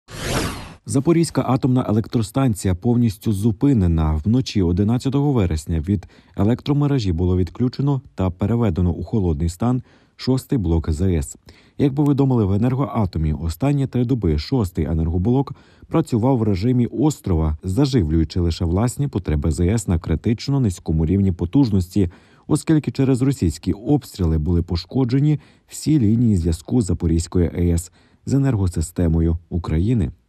Запорізька АЕС повністю зупинена, – Енергоатом
Запорізька атомна електростанція повністю зупинена. Вночі 11 вересня від електромережі було відключено та переведено у холодний стан шостий блок ЗАЕС.